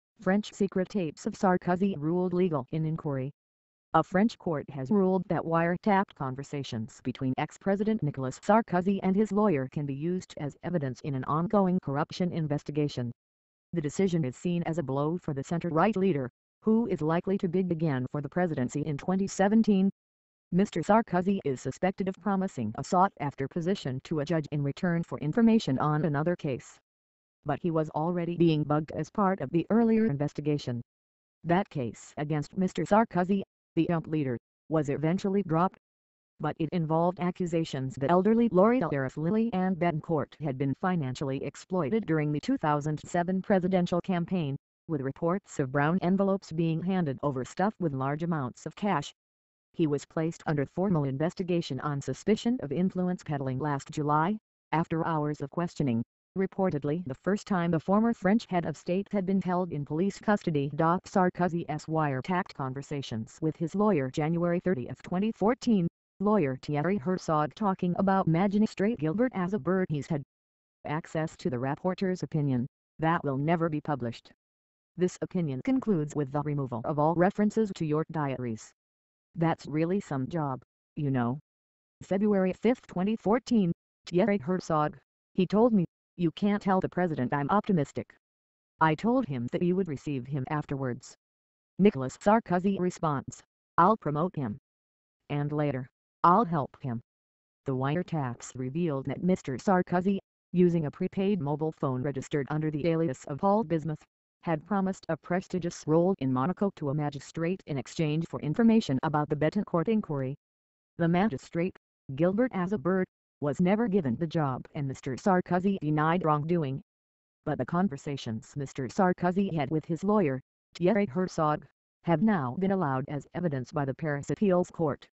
French secret tapes of Sarkozy ruled legal in inquiry - Breaking News - 07-05-2015
A French court has ruled that wire-tapped conversations between ex-President Nicolas Sarkozy and his lawyer can be used as evidence in an ongoing corruption investigation.
The decision is seen as a blow for the centre-right leader, who is likely to bid again for the presidency in 2017.
Mr Sarkozy is suspected of promising a sought-after position to a judge in return for information on another case.